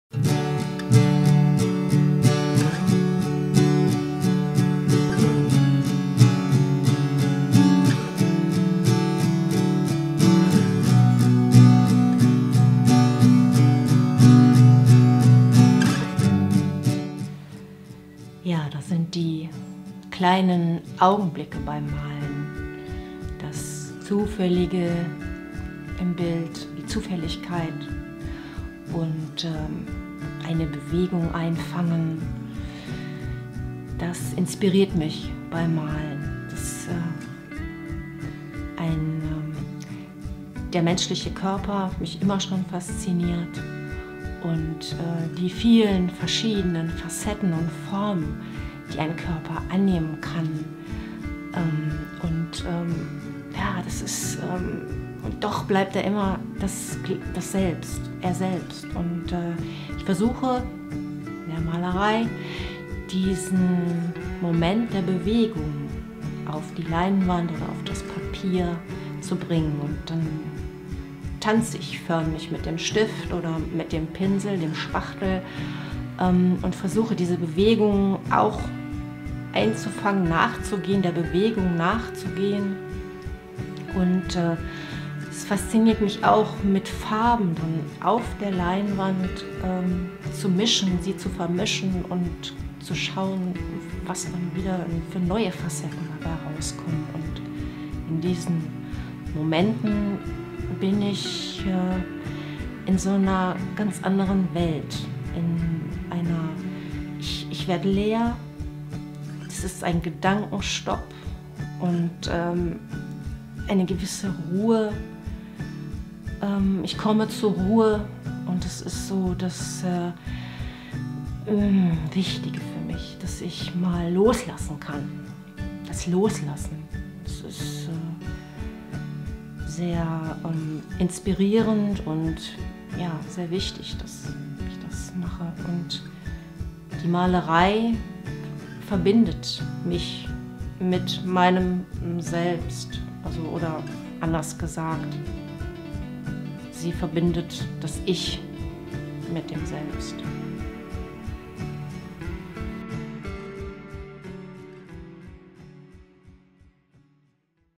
Künstlerportrait Elke Lambrecht
Künstlerportrait über die charmante Malerin und Zeichnerin Elke Lambrecht aus Witten.